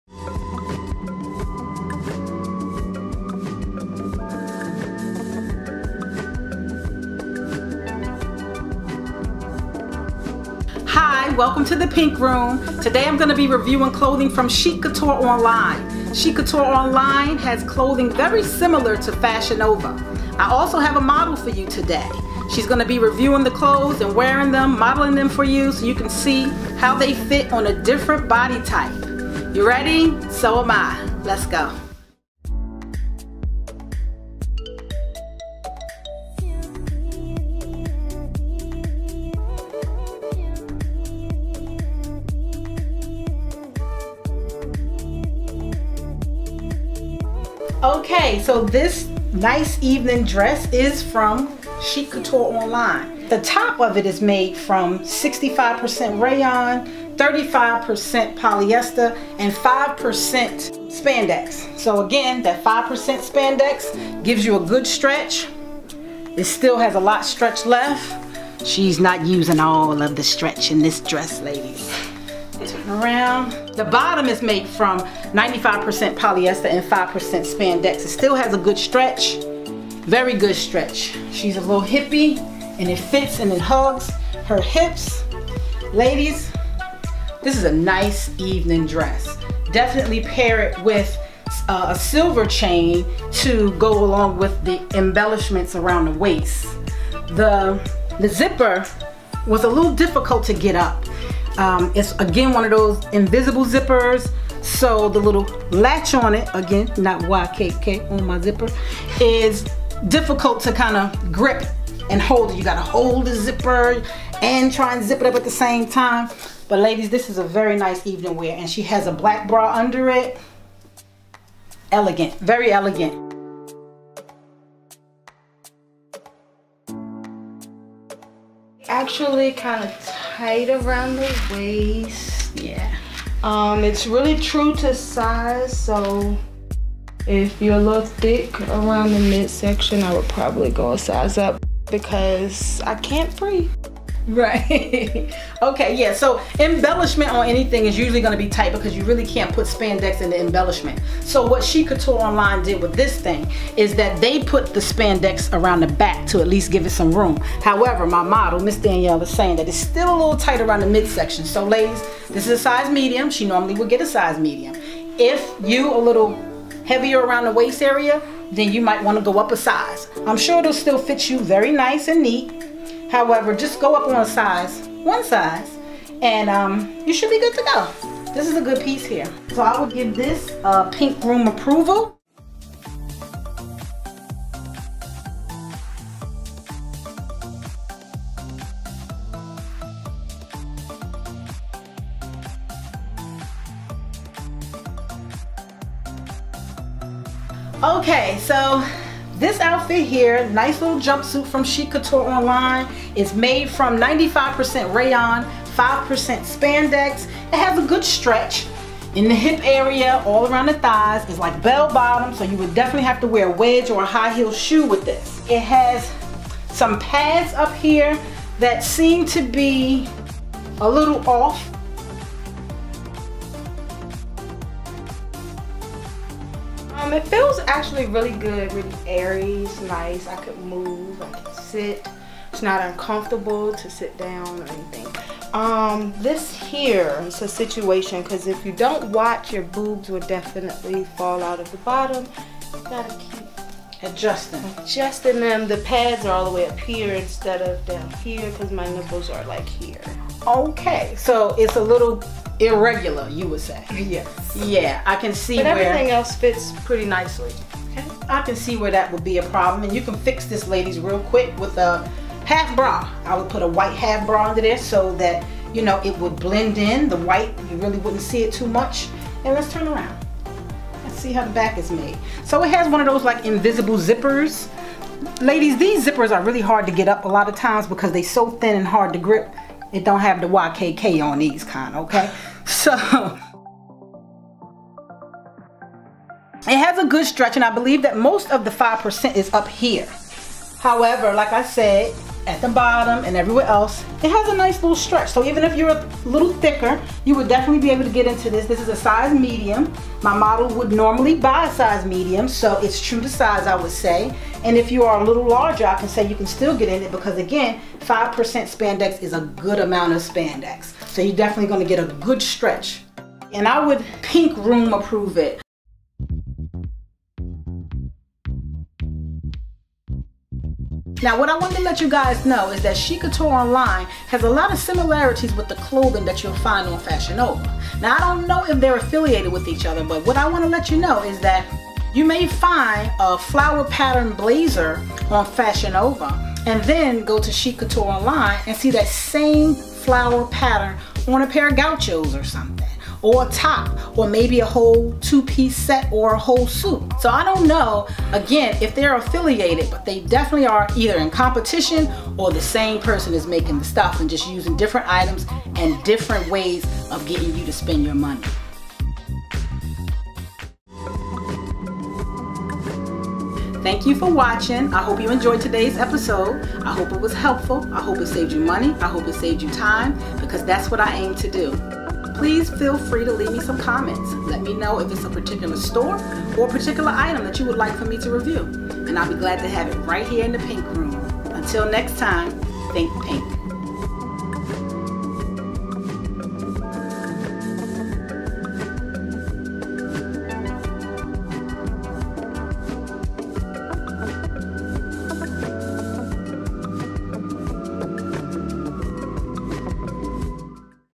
Chic Couture Online Review (S1. Ep. 6)
Hey Ladies!

On this week's episode, I invited a model, Ms. Danielle, to feature an evening maxi dress and casual chambray jumpsuit from Chic Couture Online. I have some things to point out, but the ultimate question is: Will they be Pink Room Approved or Denied? You're just one click away from The Pink Room Review!

-Pippi